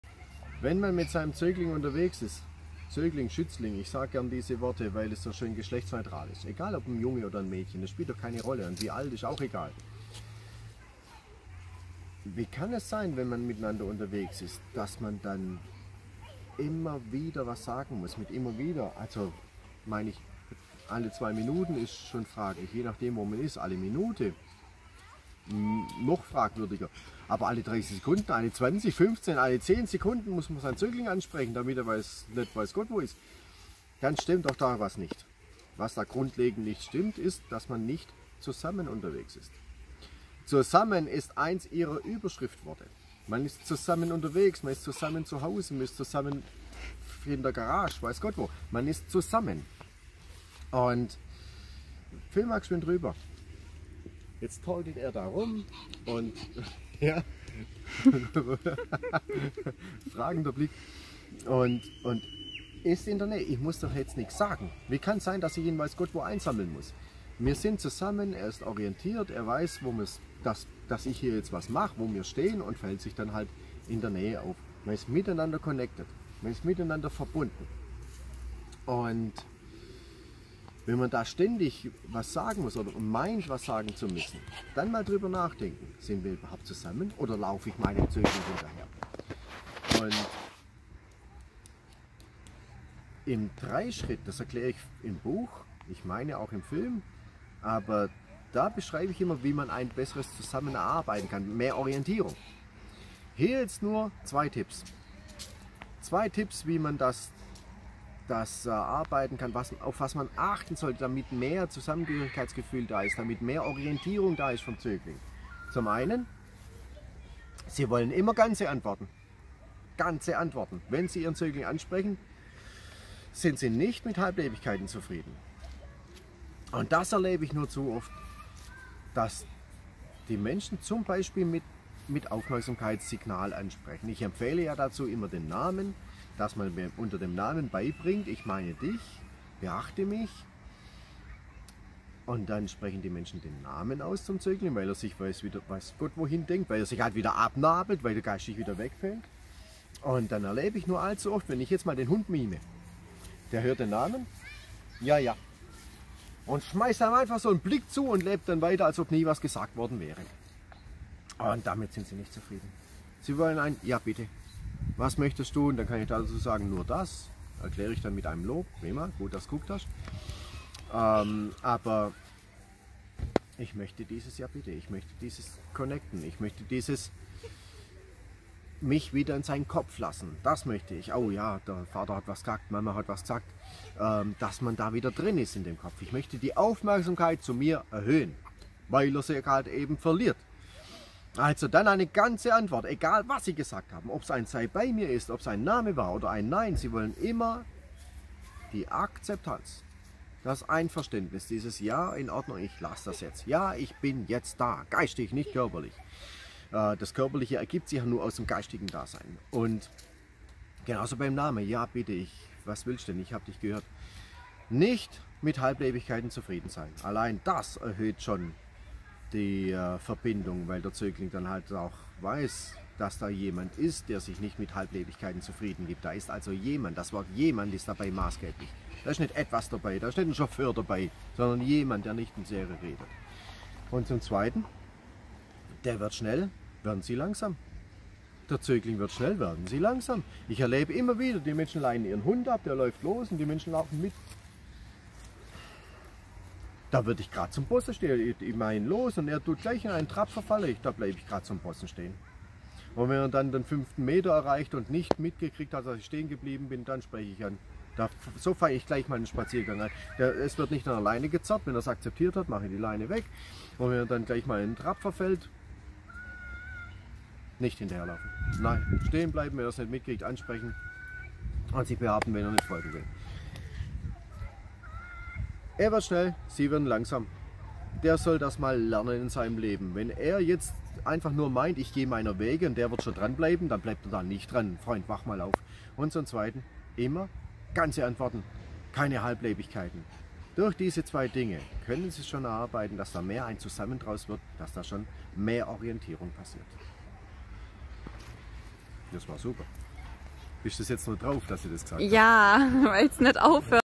Zusammen unterwegs sein
In diesem Clip erklärt Thorsten zwei wichtige Punkte, wie ihr mehr Orientierung beim Zögling erreichen könnt. Zusammen unterwegs sein, statt Chauffeur, Snackbox oder Hinterherläufer.